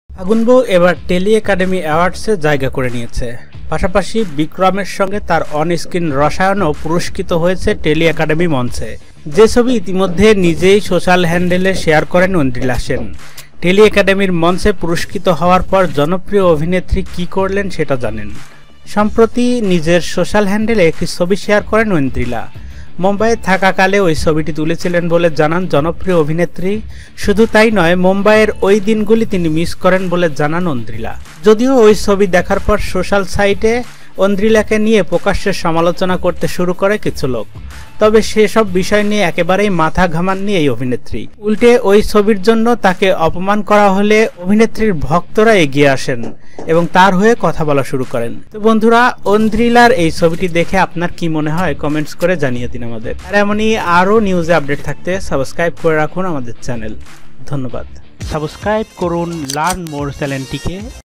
ফাগুন বউ ঔদ্রিলা সেন তার হট ছবি নিয়ে দর্শকদের সমলাচোনার শিকার। | Ondrila sen phagun bou
হট ছবি নিয়ে বিপাকে ঔদ্রিলা । ফাগুন বউ ঔদ্রিলা। Ondrila fagun bou